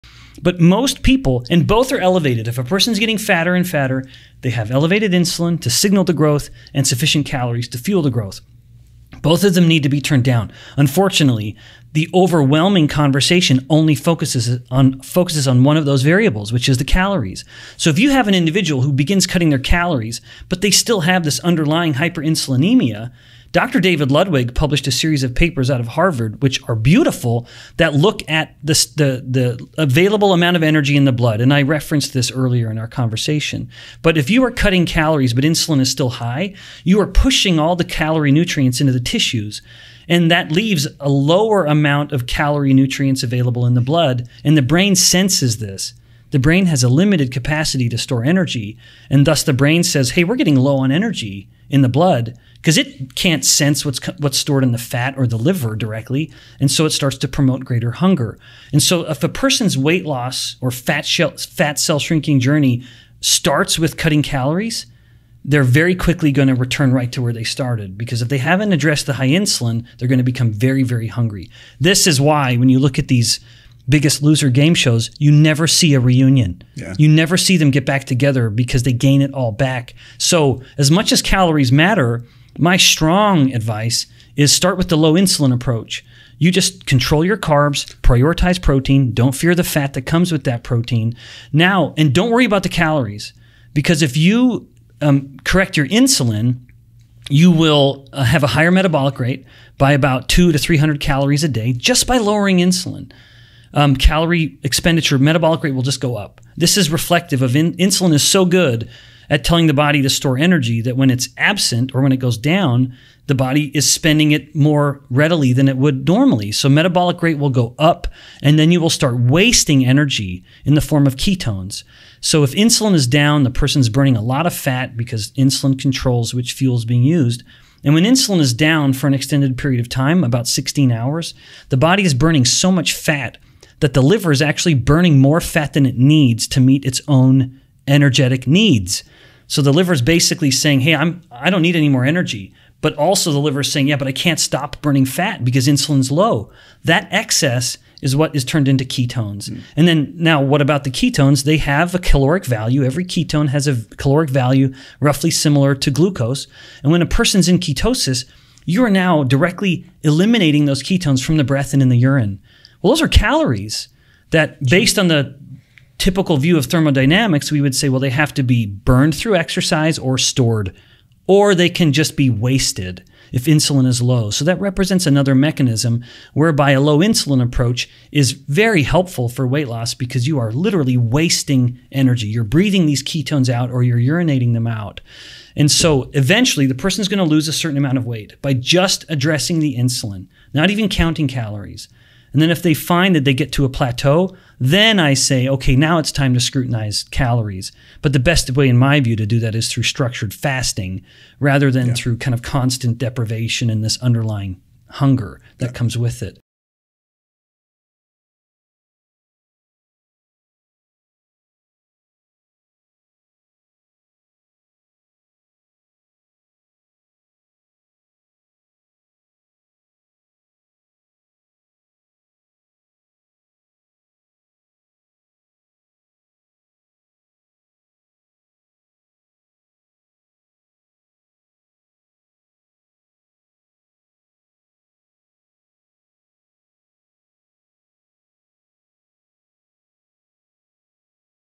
BEN BIKMAN i8 | INSULIN First; then Calories …BOTH MUST COME DOWN for FAT LOSS: START w/ INSULIN
presents episode 2143 | DR BEN BIKMAN w/Thomas DeLauer

For fat mass reduction, Both insulin & calories need lowering
-current approach is Only Focus on Calorie Reduction
That Will NOT WORK--cutting calories while still having
hyperinsulinemia [hunger will win]
David Ludwig papers: if cut calories, but insulin high—
-push calorie nutrients into tissues
-blood has lower nutrients, brain knows…
-Signals to EAT!
-will become very, very hungry
-diet will FAIL
[Biggest Loser winners gain it all back]

Effective fat mass loss: start with low insulin approach…
-control carbs
-prioritize animal protein
-don't fear the fat that comes with protein
-Don't worry about calories
With correct Insulin:
-higher metabolic rate by 300cal / day
-waste energy in ketones, with liver
  burning more than it needs
-eliminate caloric ketones in breath, urine

Continue "wasting" calories until reach plateau..
-then address calories [while hunger is not a driver]
-through structured fasting

For each video, the original Youtube links are provided above.